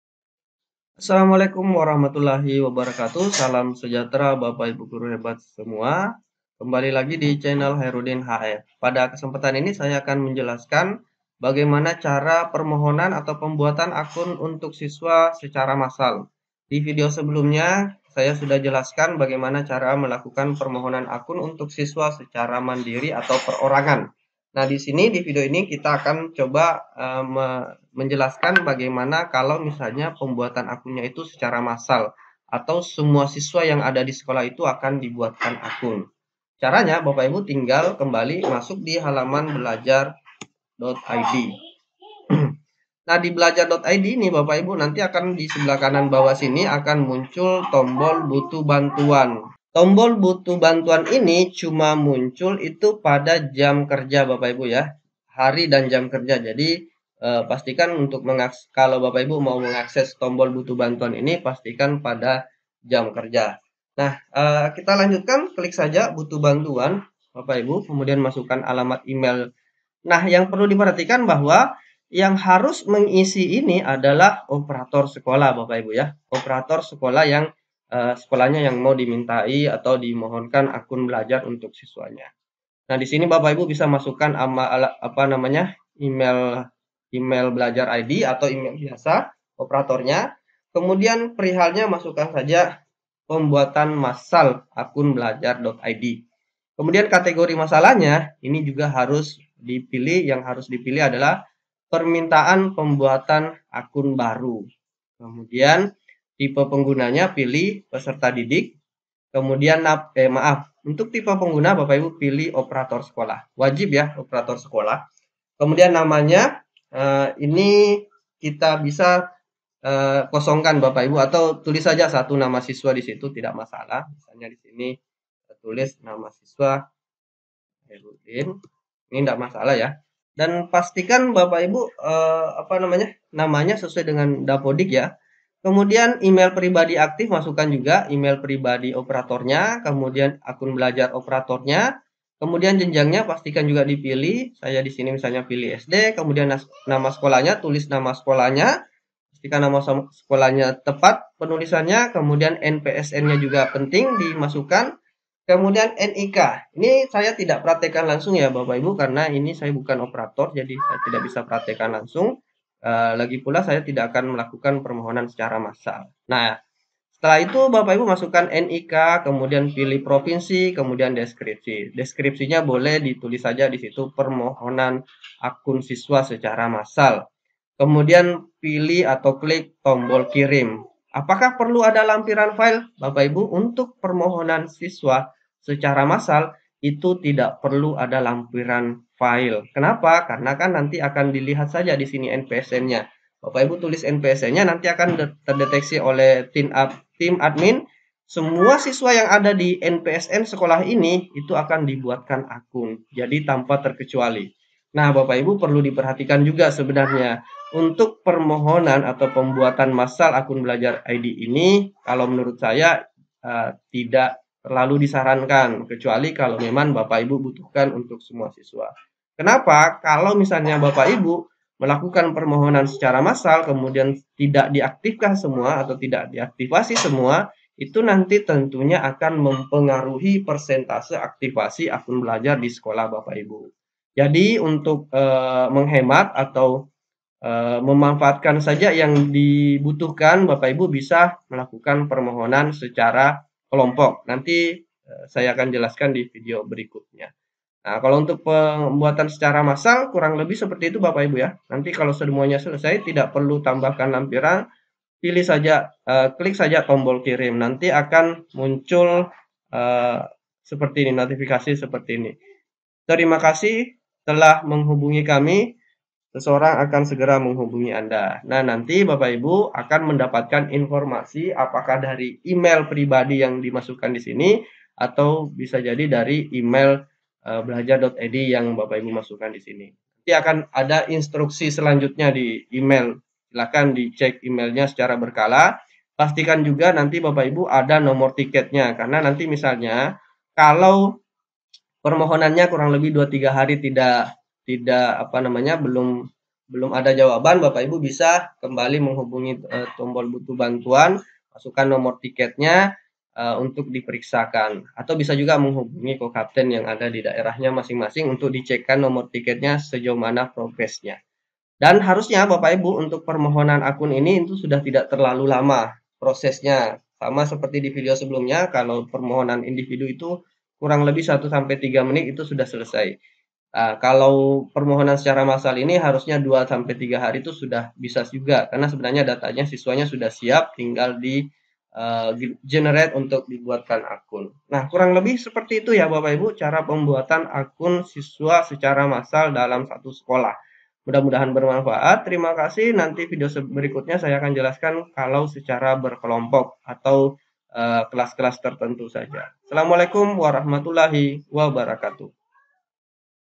Cara terbaru permohonan akun siswa secara Massal
Untuk akun siswa yang berpindah jenjang secara otomatis akan terhapus oleh sistem. Setiap satuan pendidikan yang ingin mendapatkan akun belajar untuk siswanya dapat melakukan permohoan akun dengan cara sebagai berikut